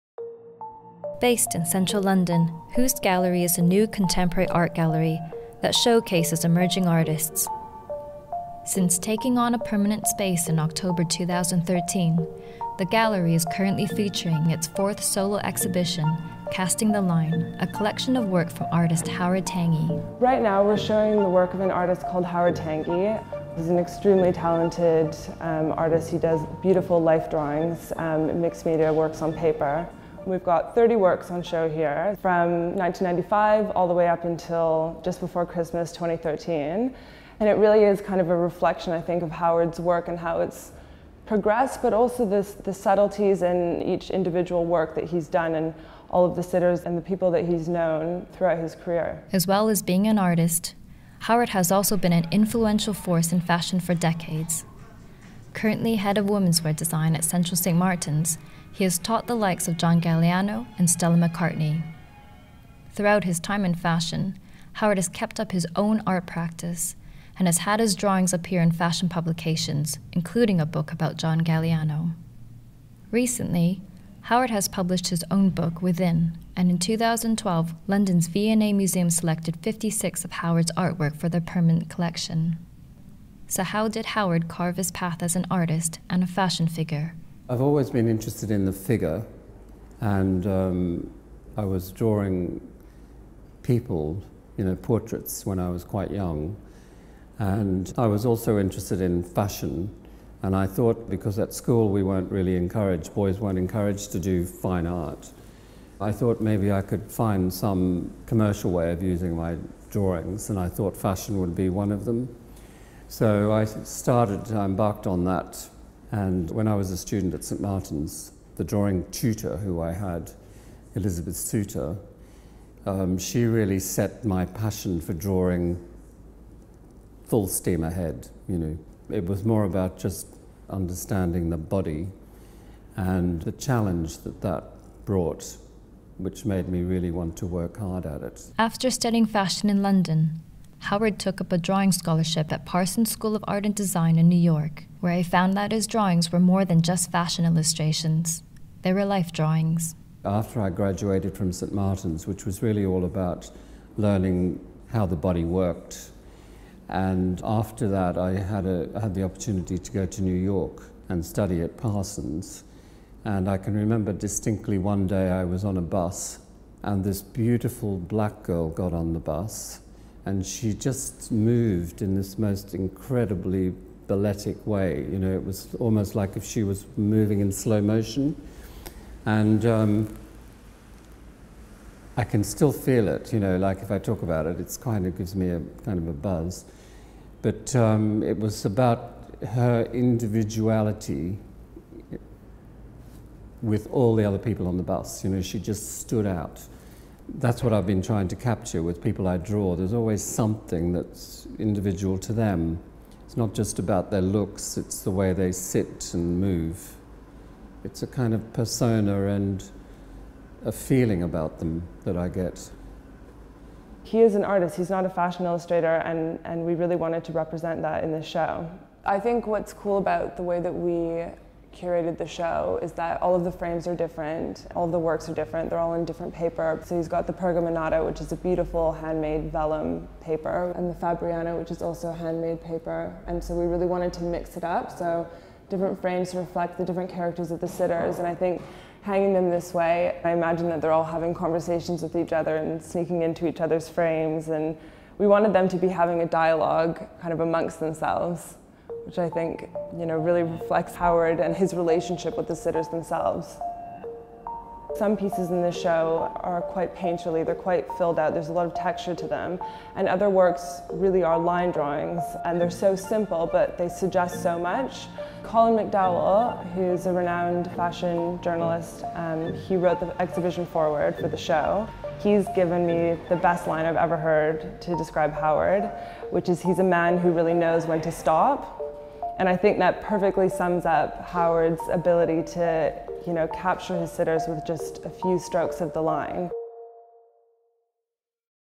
Talking art and fashion with Howard Tangye
Hus Gallery is a new, contemporary art gallery that showcases emerging artists. Based in central London, it is featuring figurative artist Howard Tangye whose work has appeared in many fashion publications.

Known for his outstanding contribution to the world of fashion as a teacher, designer and illustrator, Howard is starting to gain recognition as an artist. In 2012, London's Victoria and Albert Museum selected 56 of Howard's original works for their permanent Prints and Drawings collection and in June 2013, Howard released "Within", his first book showing 52 of his drawings.

"Casting the Line" is Howard Tangye's first solo exhibition and will be on display at Hus Gallery until 27th February 2014.